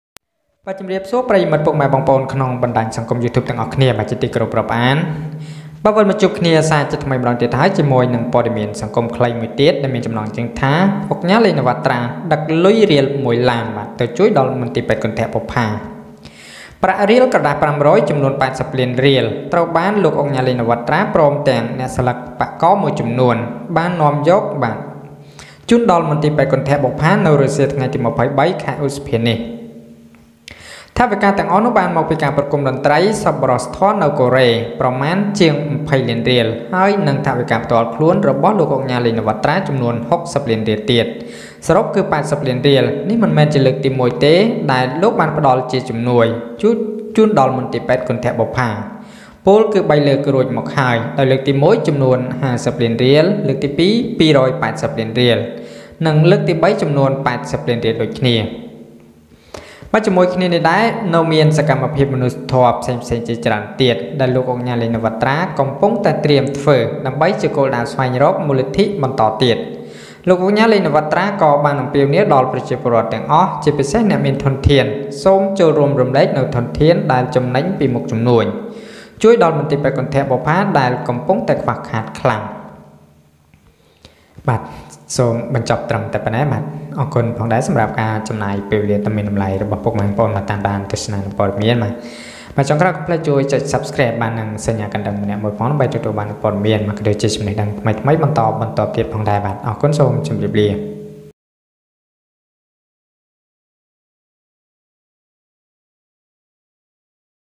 ឧកញ៉ា ឡេង ណាវ៉ាត្រា ដឹកលុយរៀលមួយឡាន ទៅជួយដល់មន្ទីរពេទ្យគន្ធបុប្ផាReal News
Luyen Vattha transported a riel to a Kantha Bopha hospital.Real News